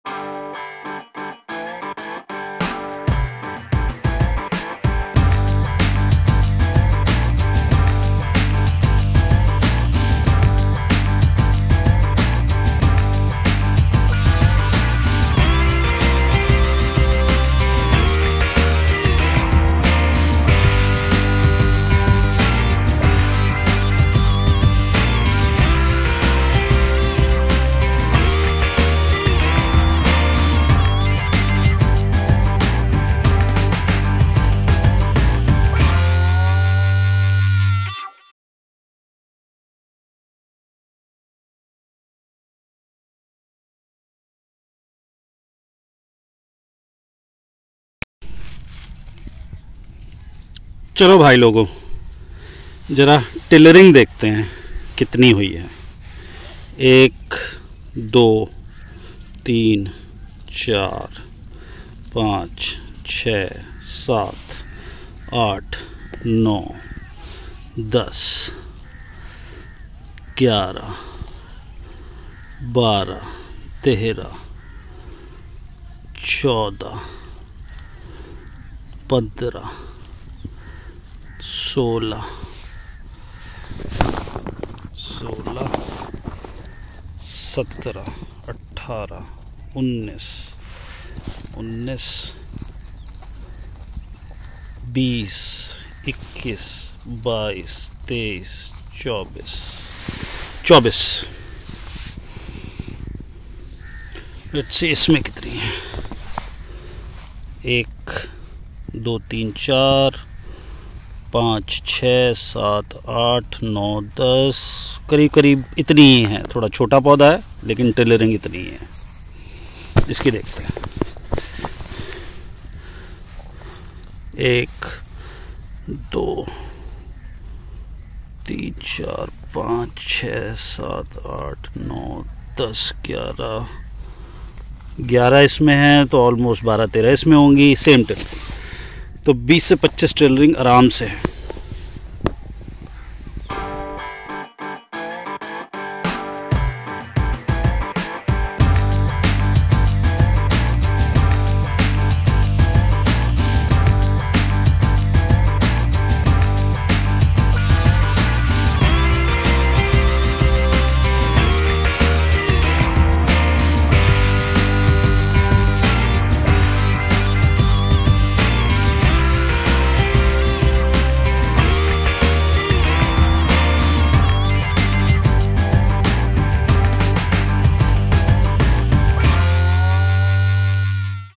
Excellent Tillering In Organic Wheat | गेहूं में जबर्दस्त फुटाव | HINDI/हिंदी | 2021
मैं अपने किसानों दोस्तों से आग्रह करता हूं कि वे हमारे पिछले वीडियो देखें कि गेहूं की सही बीज दर के साथ बुवाई कैसे की जाती है। हमने एक एकड़ में 2.8 किलोग्राम बीज लगाकर गेहूं में यह अद्भुत टिलरिंग और वृद्धि प्राप्त की।